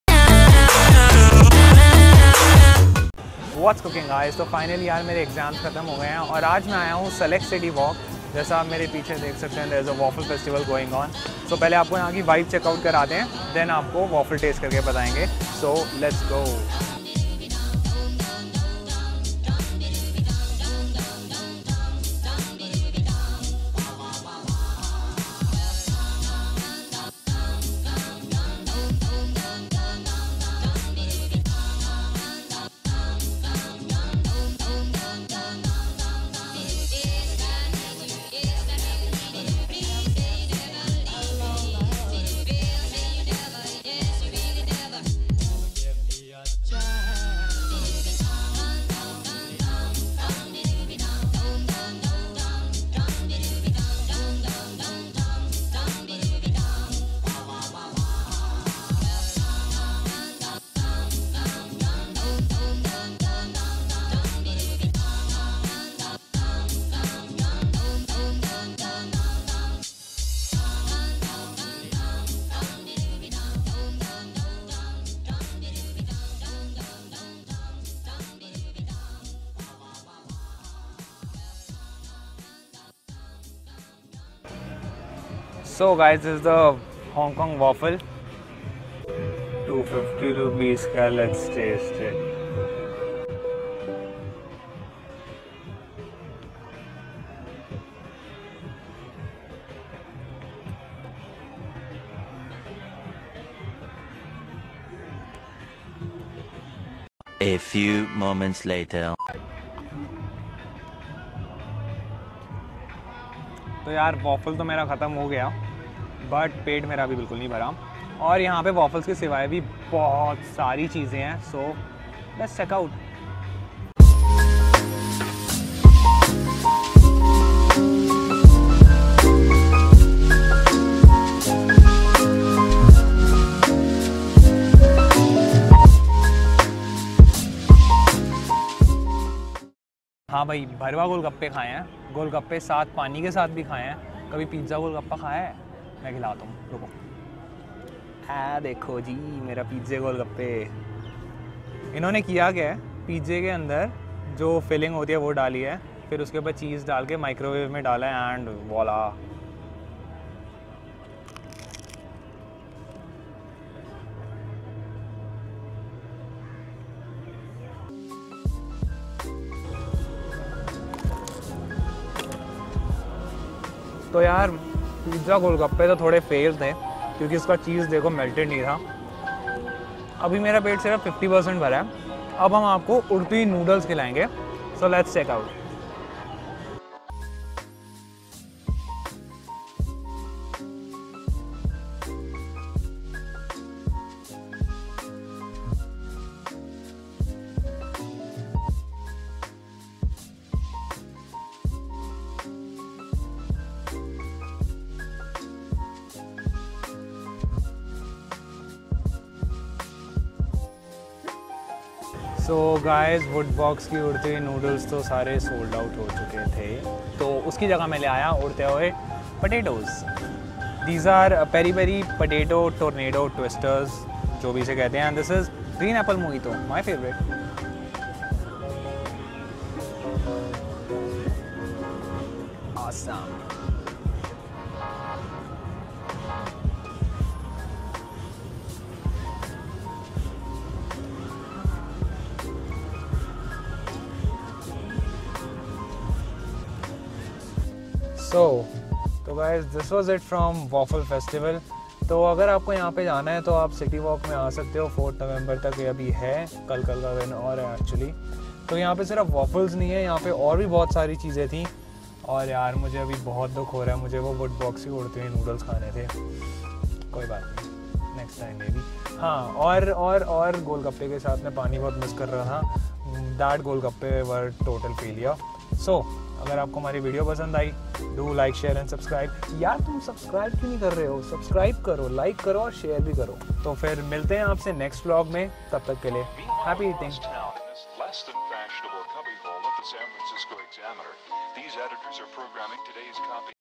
Pizza Golgappa , Maggi Pizza , Smokey Mojito and many more | WAFFLE FEST 2018
The Waffle Fest 2018 , happening at SelectCity Walk , Saket  and tomorrow is the last of it. There are a good number of waffle stalls , smokey mojitos , Pizza Golgappa , Potato Twisters , Maggi Pizza and a lot of amazing things to eat .

First we had Hong Kong Nutella Waffle , then Pizza Golgappa and then later Peri Peri Potato Twister and Smokey Mojito .

Entry - Free Entry
Dates of Fest - 2,3,4 November’18
Address - Select City Walk Mall , Saket , Near District Court , Malviya Nagar , New Delhi
Nearest metro - Malviya Nagar Station